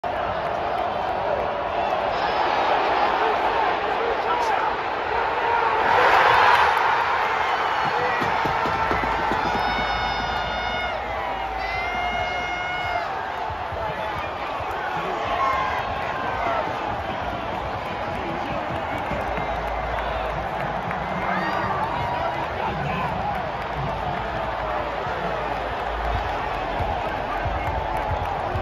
Chicago Bear's receiver DJ Moore catches a 38 yd touchdown from Justin Fields vs the Detroit Lions ￼
What an incedible game! DJ Moore catches a 38 yard pass from Justin Fields!  The Chicago Bears beat the Detroit Lions 28-13! Nothing like being at the game in person! Thanks Butko!